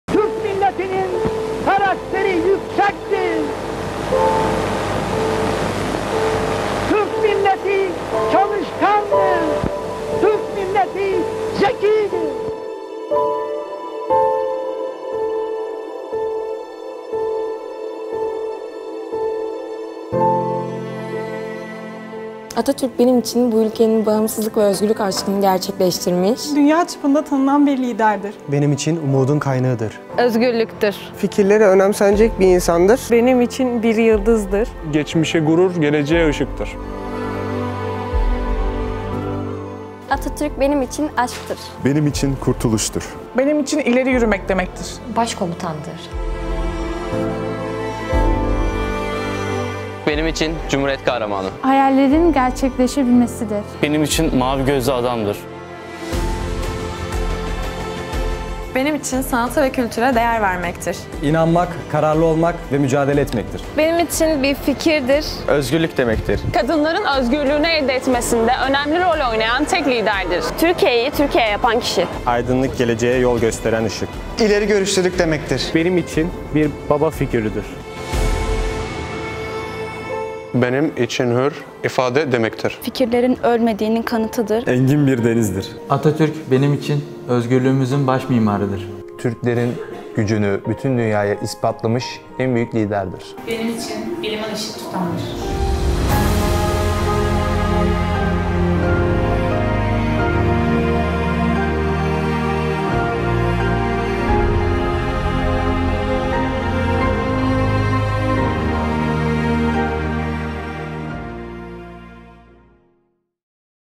O'nu anlatmaya kelimeler yetmez. 🖤
Ulu önderimiz Mustafa Kemal Atatürk’ü ebediyete intikalinin 84. yıl dönümünde saygı, özlem ve minnetle anıyoruz.